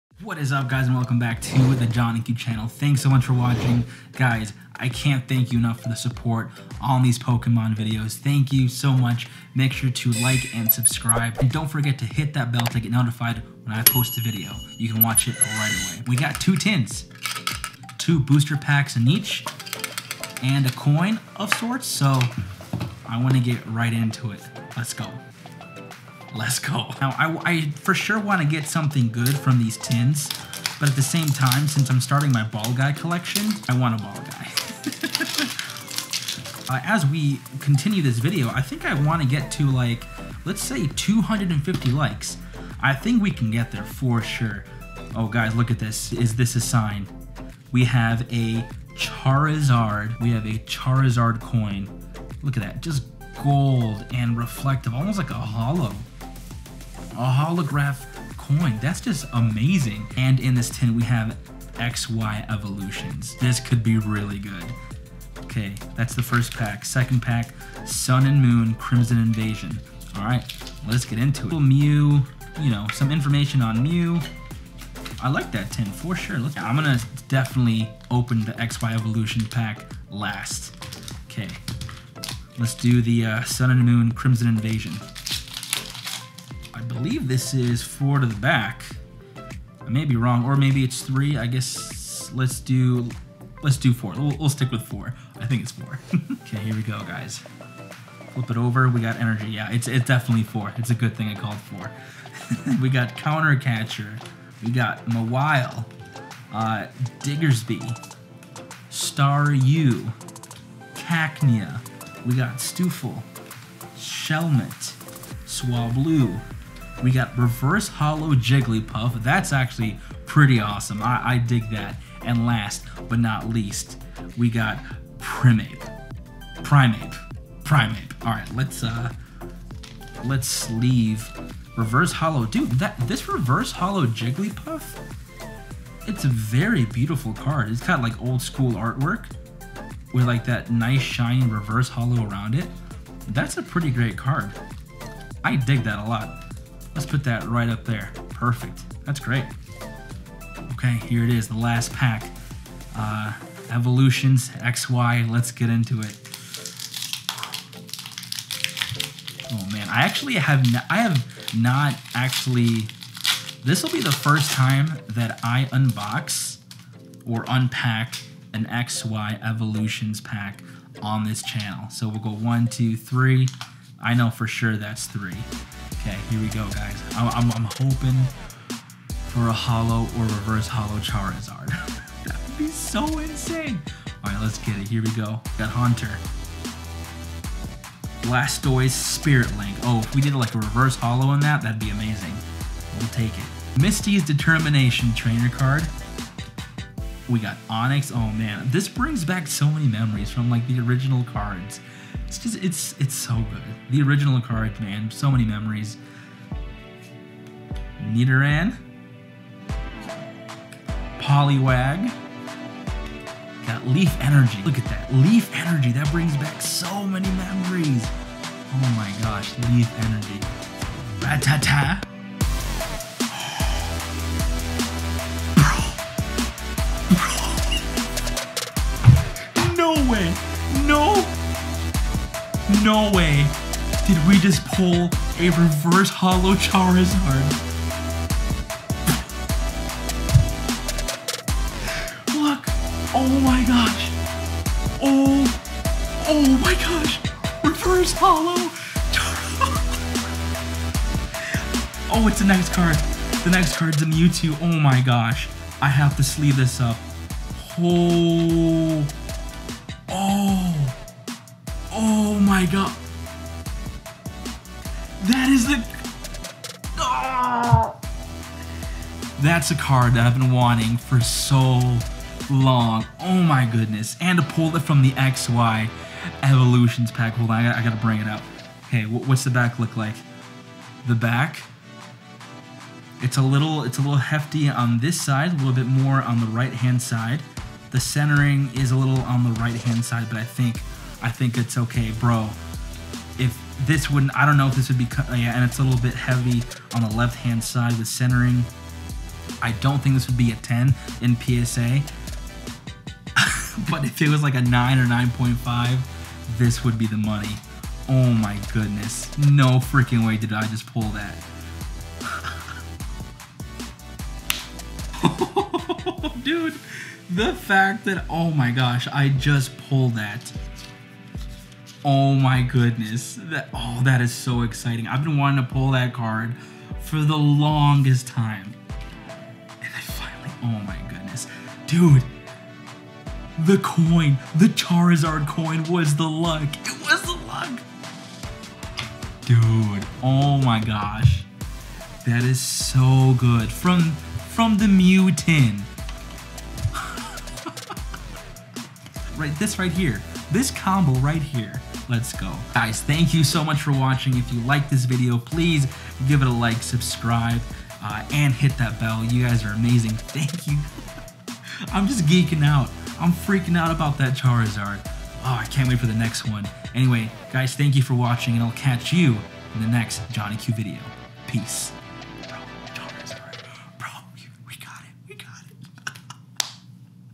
Pulling A Charizard From A Tin!
I Pulled CHARIZARD From A TIN! I FINALLY pulled it! After only 8 videos the Charizard has appeared! I pulled it from a Mew tin. Who would have thought! Let me know if I should do more XY Evolution videos! Enjoy!